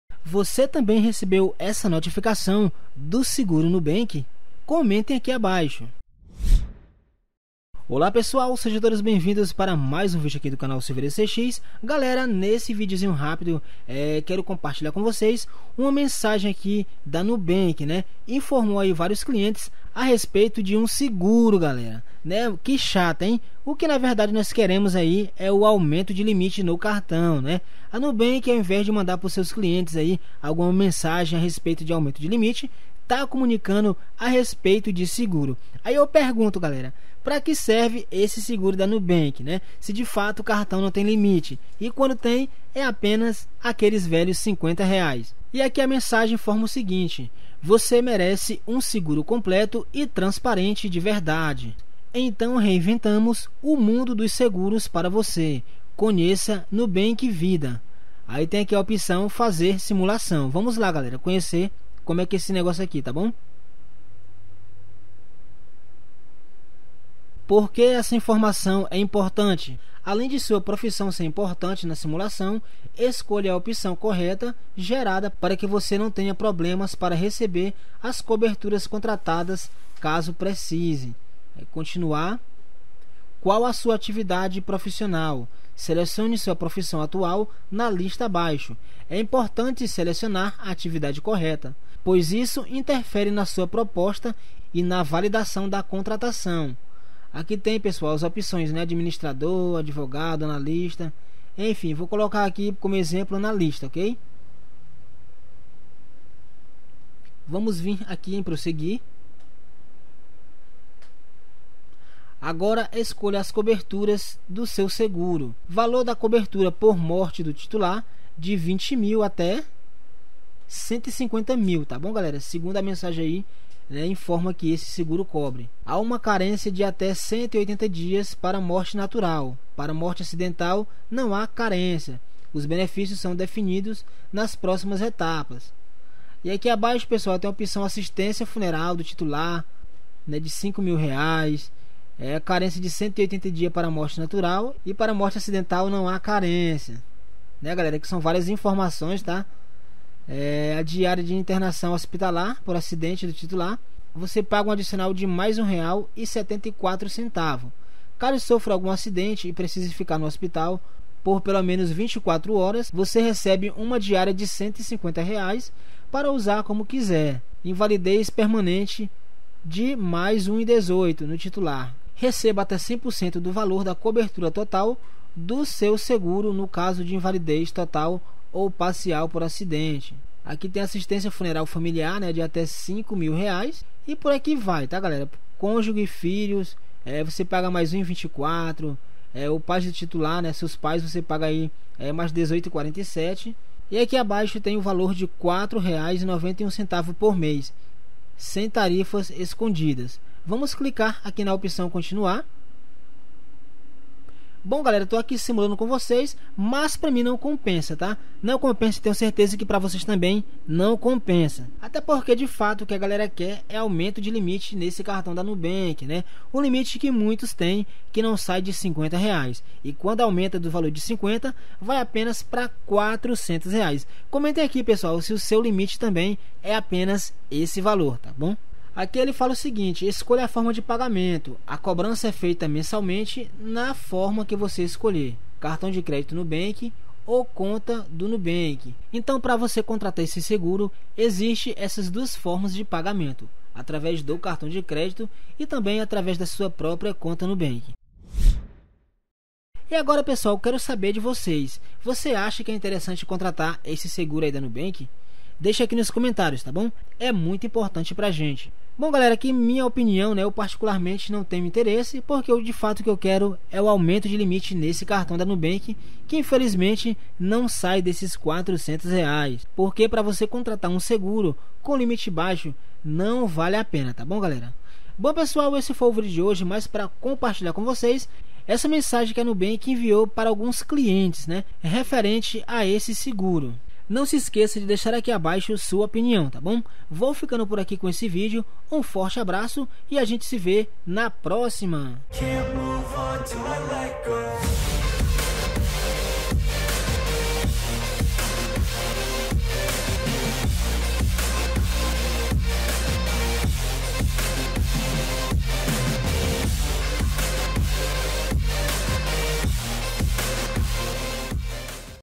❎VOCÊ RECEBEU ISSO TAMBÉM DA NUBANK? COMENTEM AQUI ABAIXO...
TODO MUNDO NA ESPERA DE AUMENTO DE LIMITE NA NUBANK E OLHA O QUE ELE MANDA PARA OS CLIENTES...